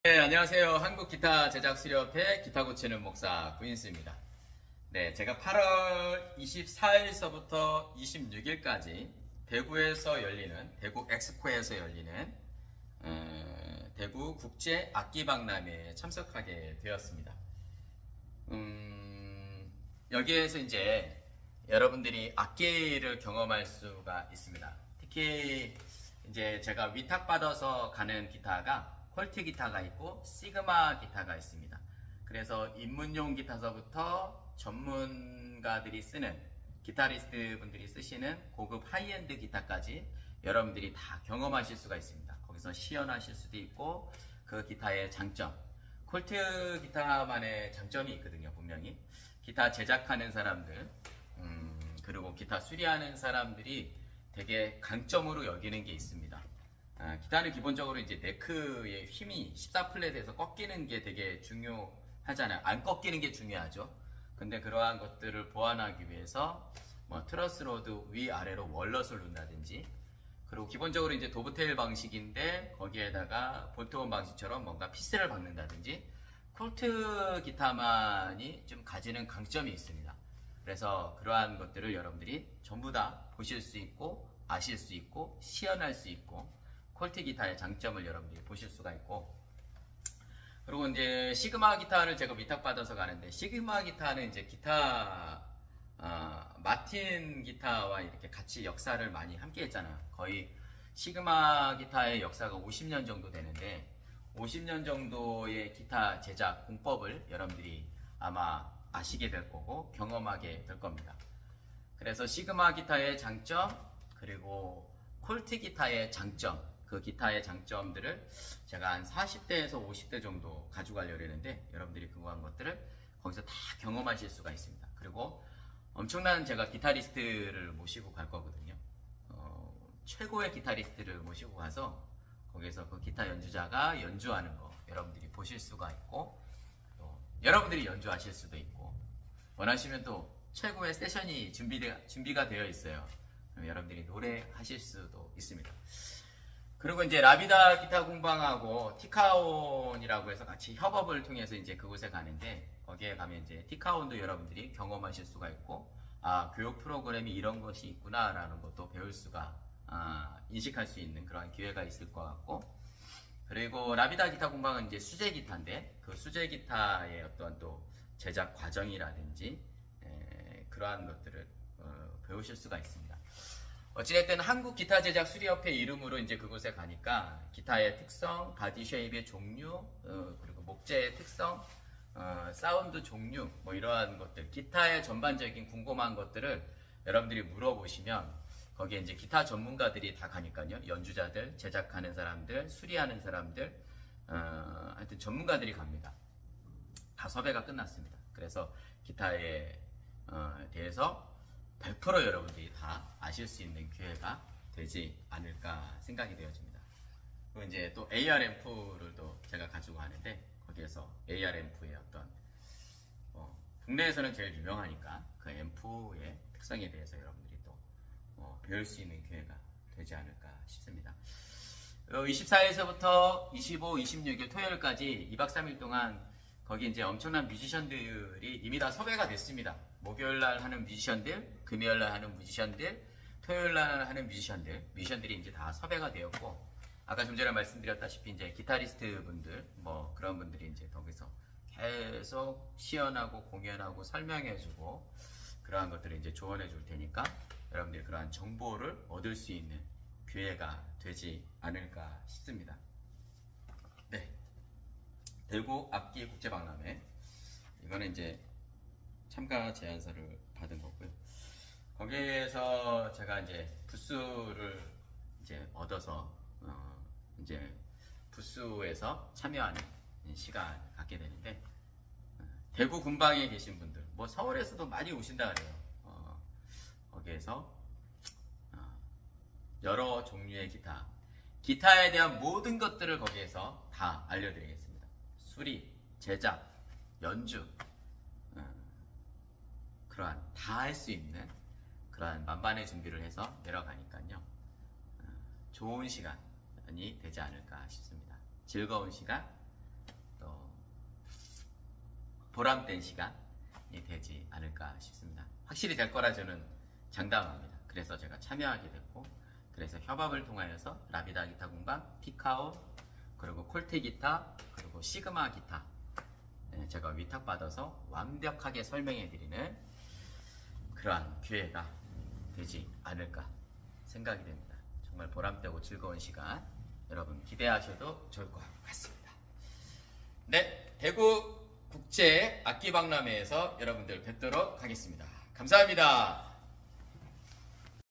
콜트기타 시그마기타 대구 국제 악기박람회 참석 (대구에서 인생기타 만나보세요♡)
한국기타제작수리협회 기타고치는목사 드림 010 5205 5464